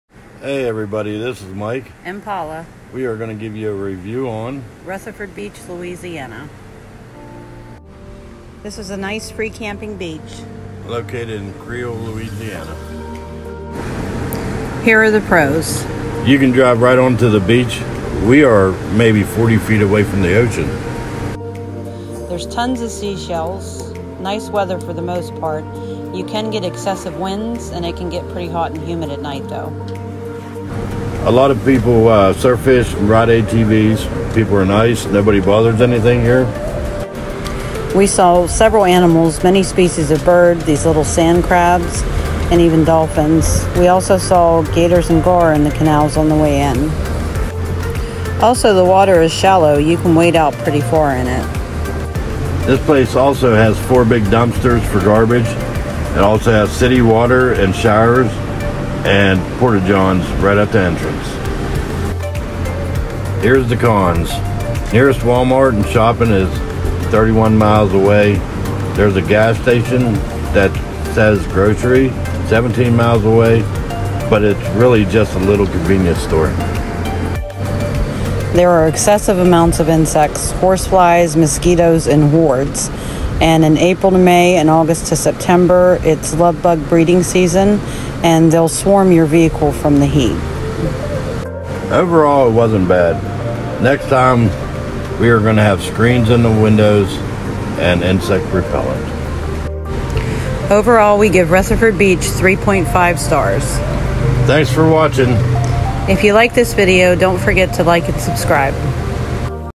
Rutherford Beach Louisiana
Review of free camping right on the beach at Rutherford Beach Louisiana.